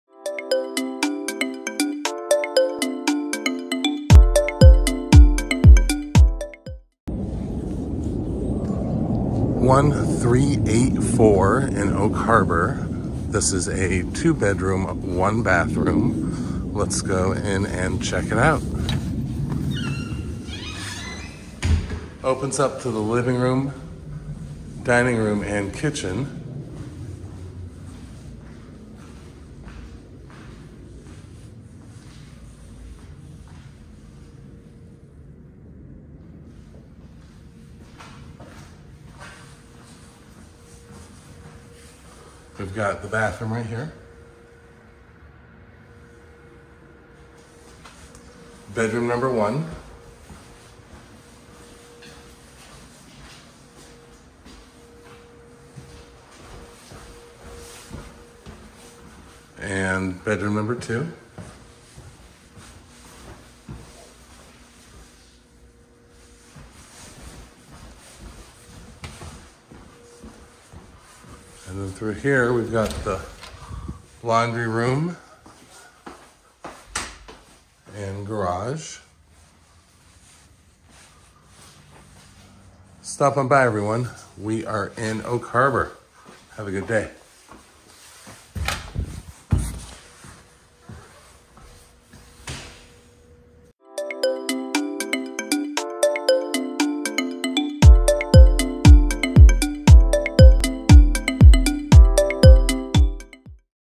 Oak Harbor Homes for Rent 2BR/1BA by Oak Harbor Property Management
This house features approximately 926 square feet of living space, 2 beautiful bedrooms, and and an attached garage. It boasts s a good-sized living area that is drenched in natural light. Next to the living area is the kitchen and dining area where you can prepare nice meals. The bedrooms are ample in size, while the bathroom is well-appointed. Other features include lovely floors throughout and a laundry room. This home is a great find!

Looking for a property manager in Oak Harbor? We do property management!

100 2nd Ave S Ste 200, Edmonds, WA 98020